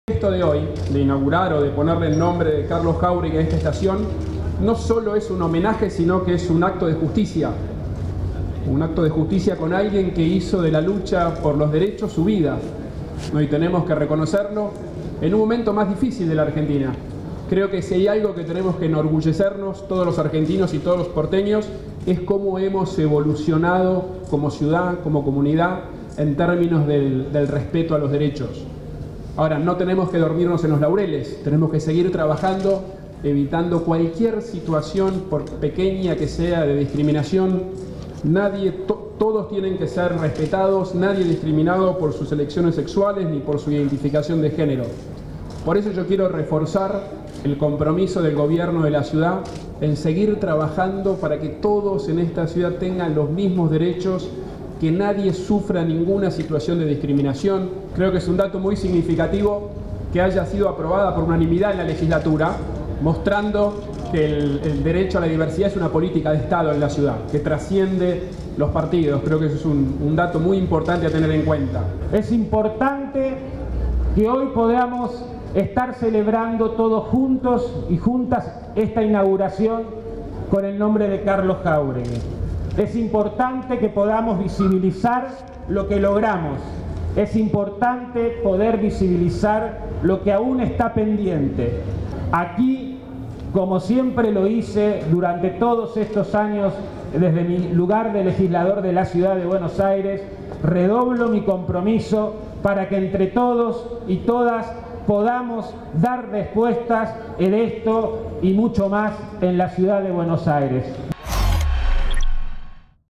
Rodríguez Larreta inauguró nuevo nombre de estación de Línea H “Santa Fe – Carlos Jáuregui"
El jefe de Gobierno de la Ciudad de Buenos Aires, Horacio Rodríguez Larreta, inauguró esta mañana el nuevo nombre de la estación de la línea H del Subte “Santa Fe – Carlos Jáuregui”, la primera en el mundo que lleva el nombre de un militante de la comunidad LGBTIQ.

La iniciativa, impulsada por los legisladores Maximiliano Ferraro de la Coalición Cívica, Carlos Tomada y Pablo Ferreyra, del Frente para la Victoria, fue votada en diciembre pasado en la Legislatura porteña y constituye un espacio de visibilización del colectivo LGBTIQ (lesbianas, gays, bisexuales, transexuales, intersexuales y queers) que durante mucho tiempo ha sido un grupo vulnerado en derechos en nuestra sociedad.